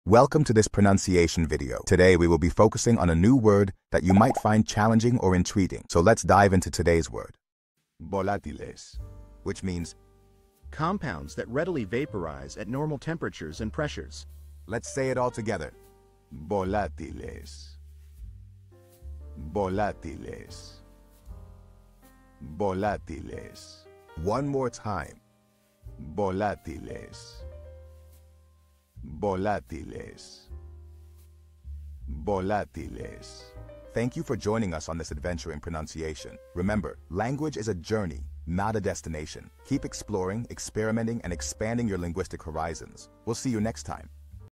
How to pronounce Volátiles
Master the Pronunciation of 'Volátiles Which means volatile' - which means : Compounds that Readily Vaporize AT Normal Temperatures and Pressures. 📝 with @PronunciationSpanish 🎓🗣️ - [Your Guide to Spanish]

Learn to pronounce 'Volátiles' perfectly in Spanish with our easy and engaging tutorial by @PronunciationSpanish! 📖📽️ In this comprehensive video, we provide you with native speaker examples 🗣️👂 and phonetic breakdowns 🎵 to ensure you get the pronunciation just right.

How to say Volatile in Spanish